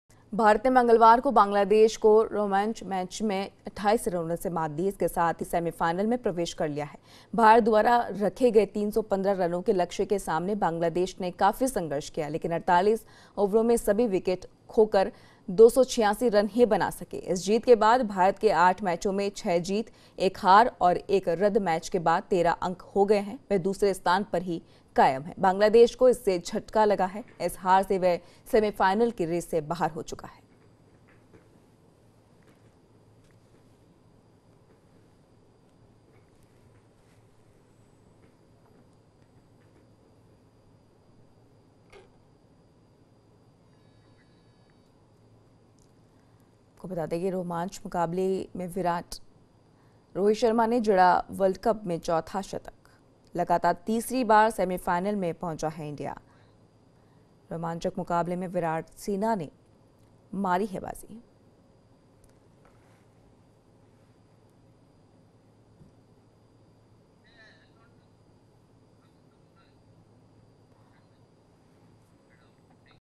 ICCWorldCup2019 - रोमांचक मुकाबले में विराट सेना ने मारी बाजी - India reach semifinals
Sports News : रोमांचक मुकाबले में विराट सेना ने बाजी मार ली - India reach semifinals for the third consecutive time - ICCWorldCup2019 - NewsIndia | WorldCup

लगातार तीसरी बार सेमीफाइनल में पहुंचा इंडिया. रोहित शर्मा ने जड़ा वर्ल्डकप में चौथा शतक. क्रिकेट वर्ल्ड कप के रोमांचक मैच में विराट की सेना ने मरी बाजी. टीम इंडिया ने बाजी मार ली